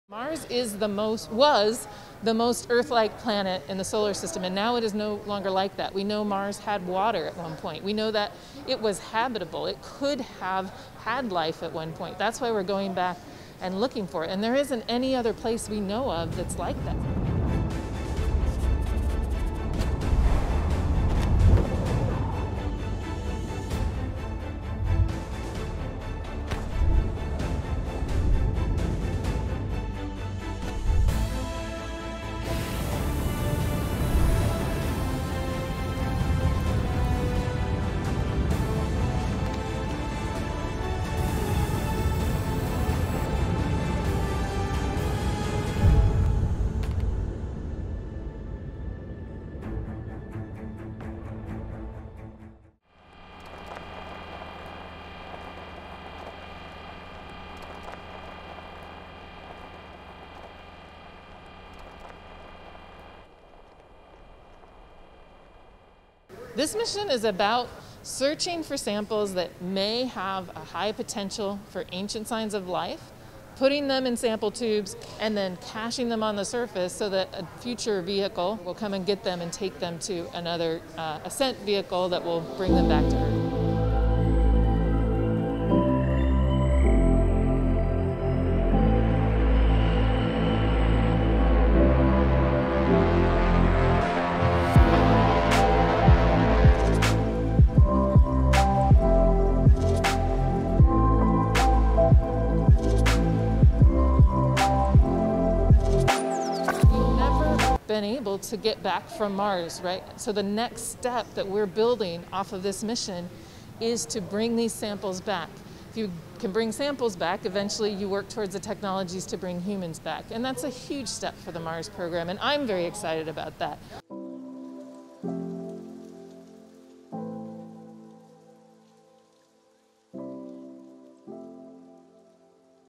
Mission Perseverance : un robot de la taille d'une voiture pour déterminer si la vie a existé sur
Après sept mois de voyage, le rover Perseverance de la Nasa tentera d’atterrir sur Mars. La manœuvre, ultra-périlleuse, marquera le début d’une quête de plusieurs années à « la recherche d’échantillons qui pourraient nous montrer des traces de vie ancienne » explique Jennifer Trosper, l’une des responsables de la mission. Les tubes seront ensuite conservés jusqu’à ce qu’une mission ultérieure soit capable de revenir les chercher, aux alentours des années 2030.
« Nous n’avons pas encore réussi à repartir de Mars. La prochaine étape, et cette mission va nous aider à y arriver, c’est de pouvoir ramener les échantillons. Une fois que vous savez faire ça, alors vous pouvez travailler sur des technologies permettant de faire revenir des humains. C’est une étape énorme pour le programme Mars, je suis impatiente », poursuit Jenifer Trosper.
Un autre objectif de la mission sera de parvenir à faire voler au-dessus du sol martien un engin motorisé pour la première fois. Un autre objectif est de préparer de futures missions humaines, en faisant l’expérience de production d’oxygène directement sur place.